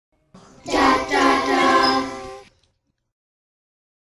4 Mawhinney Singing
Grade 4 students singing multi-part harmonies